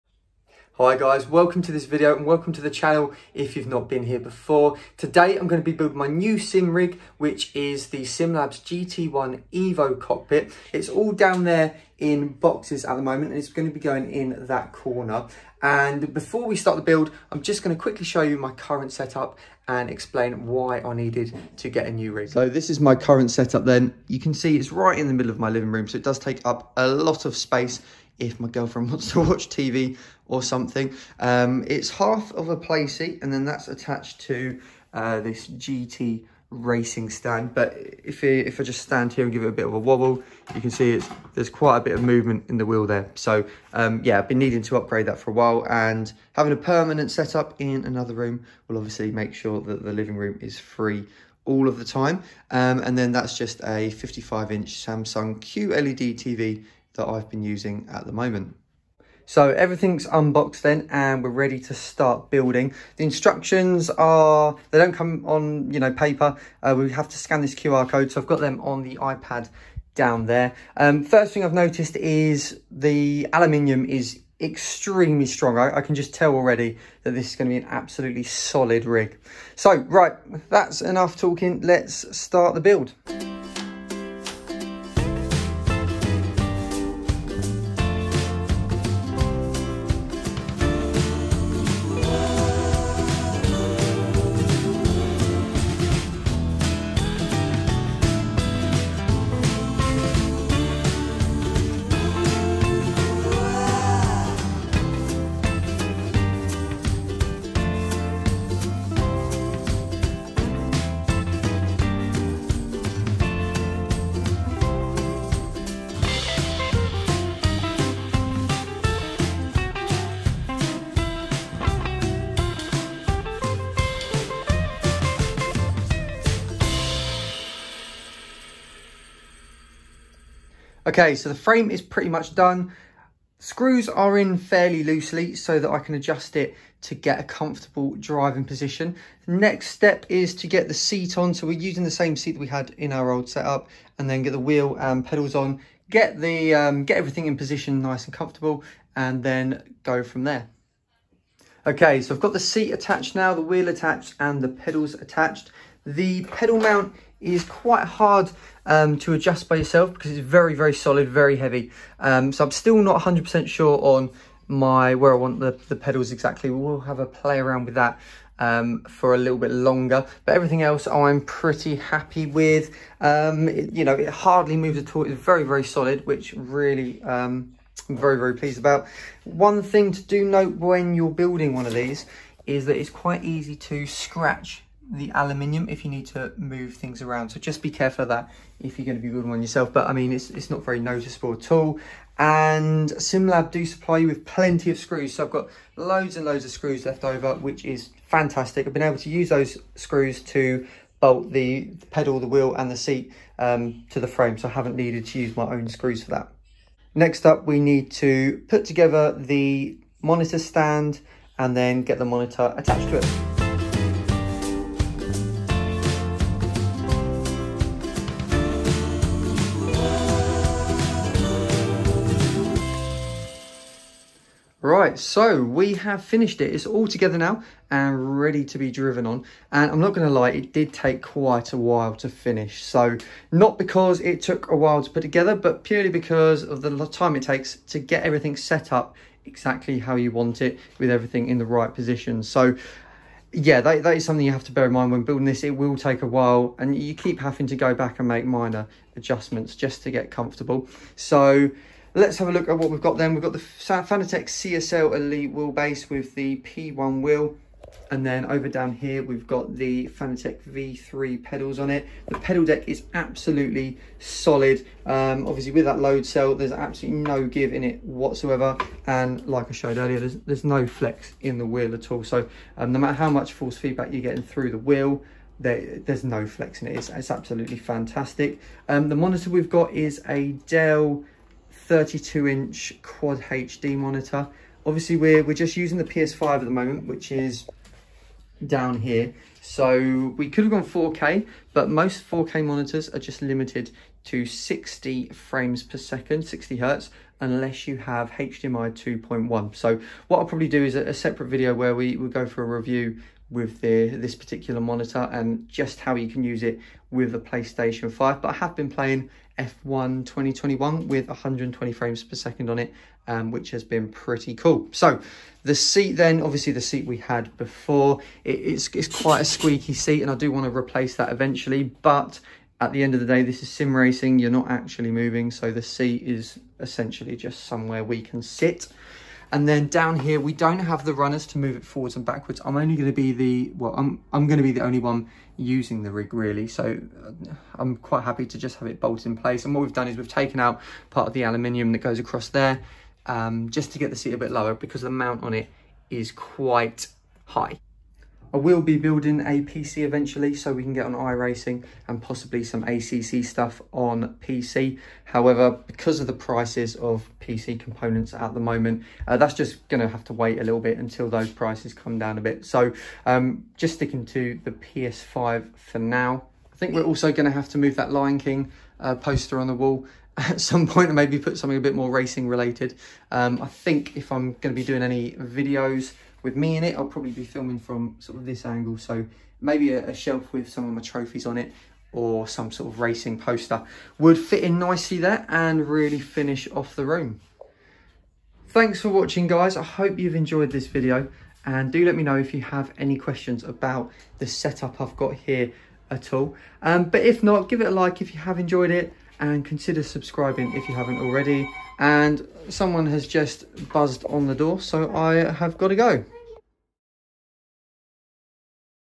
Sim-Lab GT1-Evo Sim Racing Cockpit - Build and Short Review + Sim Rig Tour!
I've finally upgraded my setup with a Sim-Lab GT1-Evo Cockpit! I have been needing something more solid for quite a while as my previous wheel stand would wobble all over the place! I hope this video is helpful for anyone considering upgrading to an aluminium rig - I would highly recommend.

Full set up:

PS5
Wheelbase: Fanatec CSL Elite
Wheel: Fanatec P1
Pedals: Fanatec Clubsport V3 pedals
Rig: Sim-Lab GT1-EVO cockpit + Monitor Stand
Monitor: Dell S3220DGF 31.5 inch QHD

100% of all donations are used for improving the channel and supporting real-world racing